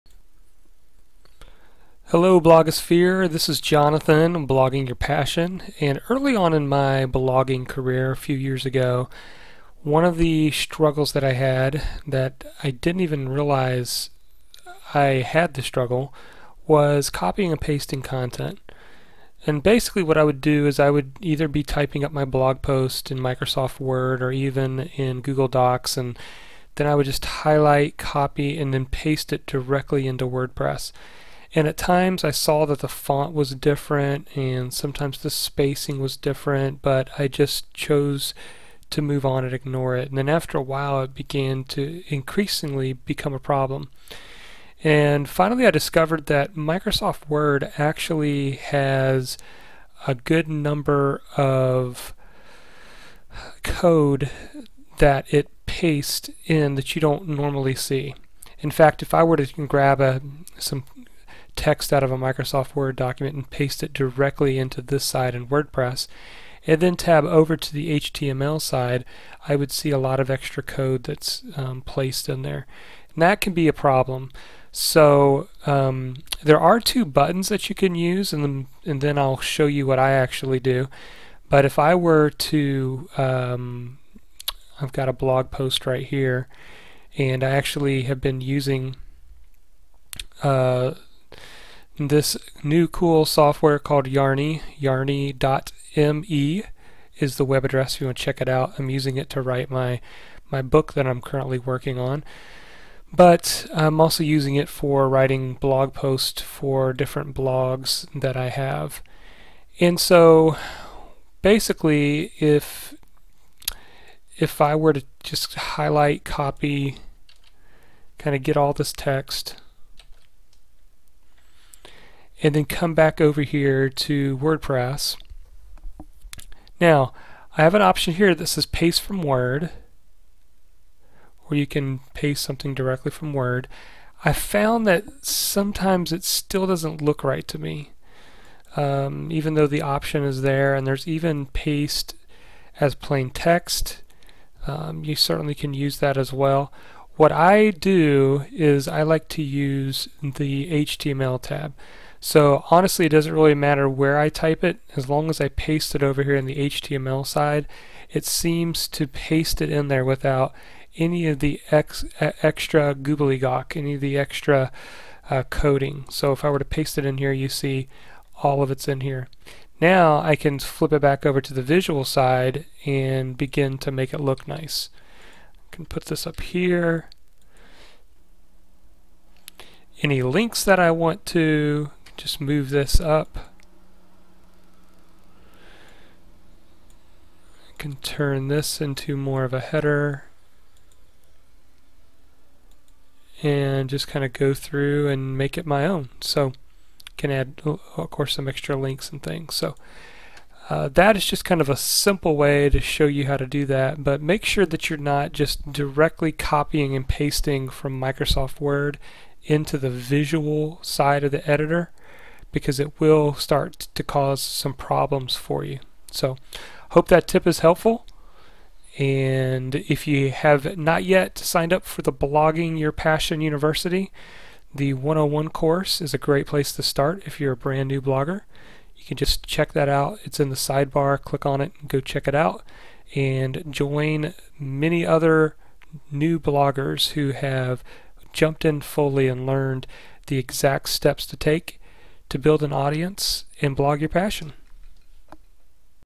How to Properly Copy and Paste Your Content into Wordpress
When I first started blogging with Wordpress, I would often type my blog post in Microsoft Word and then transfer it over into Wordpress. This caused issues with the formatting inside Wordpress. Watch this video to see how I solved the problem for good!